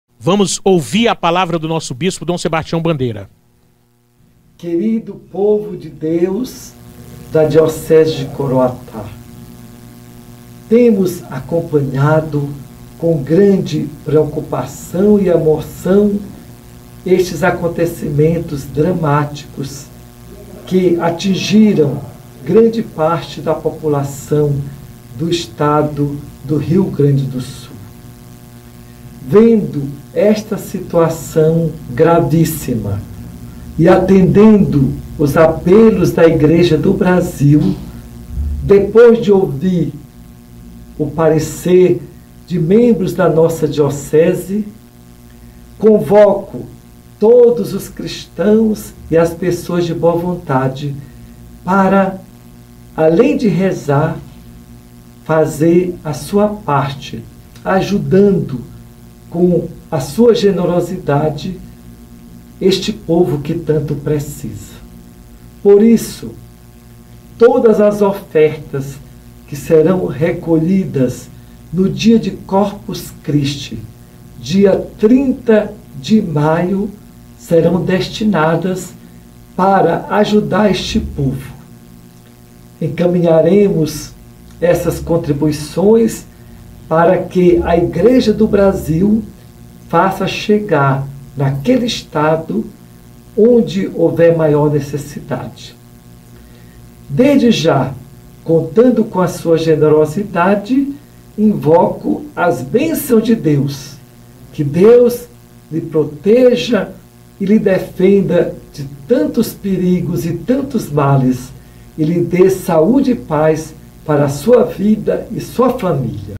DIOCESE DE COROATÁ INICIA CAMPANHA PARA ARRECADAR DOAÇÕES PARA O RIO GRANDE DO SUL
ANTONIO SILVA
ELTON ANTONIO
RADIO EDUCATIVA